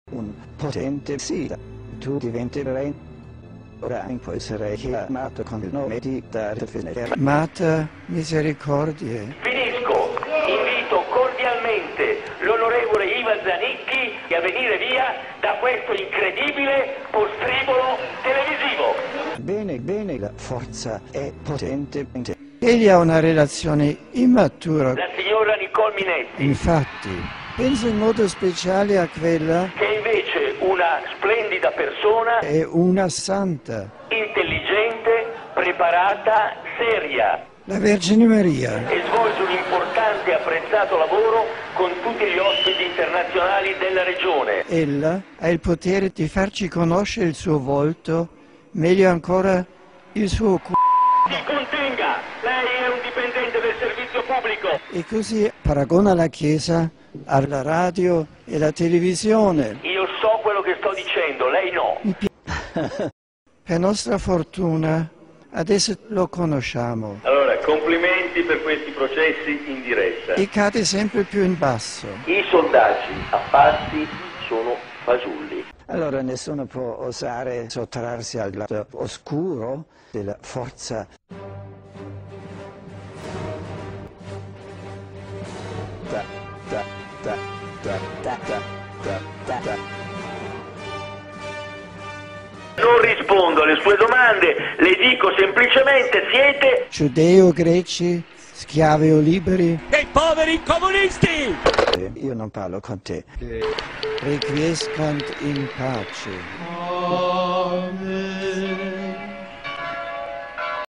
colloquio tra il papa e berlusconi
creato da trenorapido (PSN)
con i riferimenti in questo video non intendo offendere nessuno.é satira fatevi una risata!!! e mi raccomando commentate.